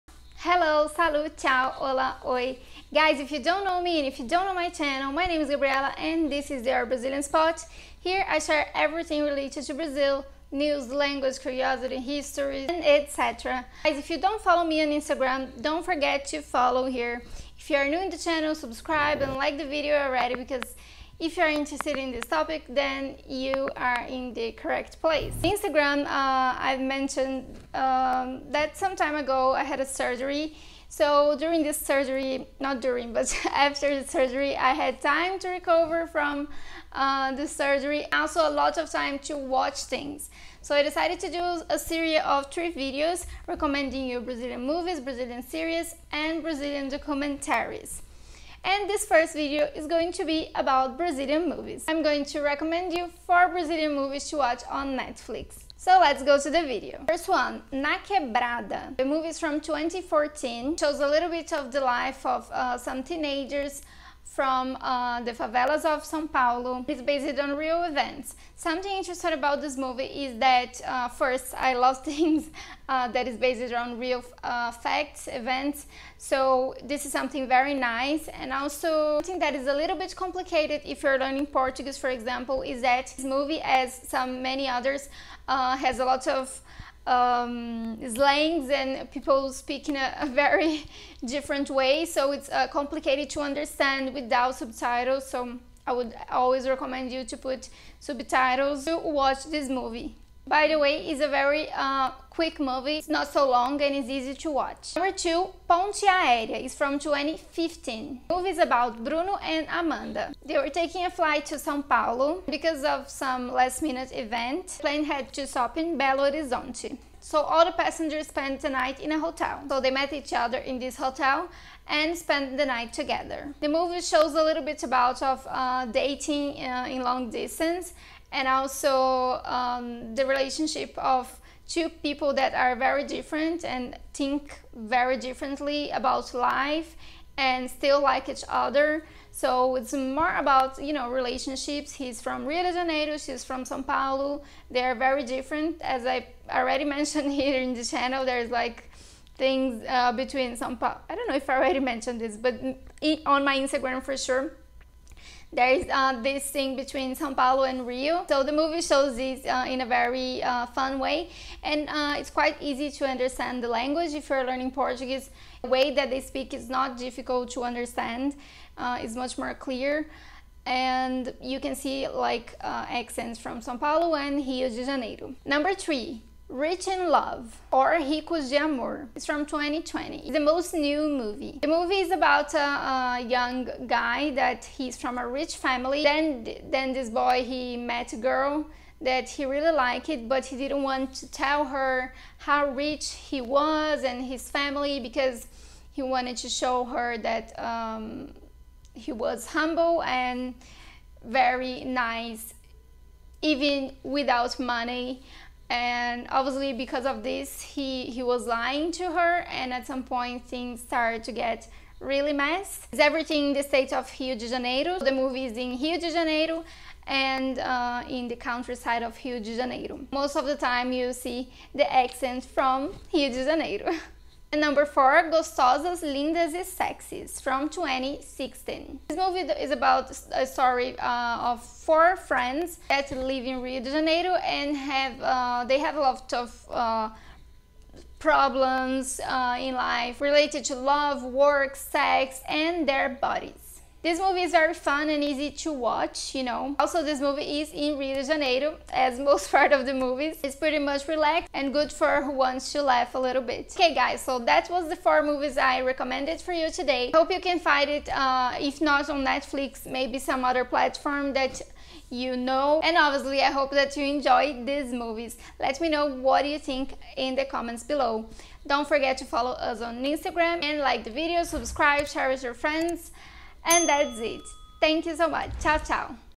4 Brazilian Movies To Watch On Netflix
4 Brazilian Movies To Watch On Netflix

More recommendations to watch on Netflix, I hope you enjoy all of them. Let me know your opinion!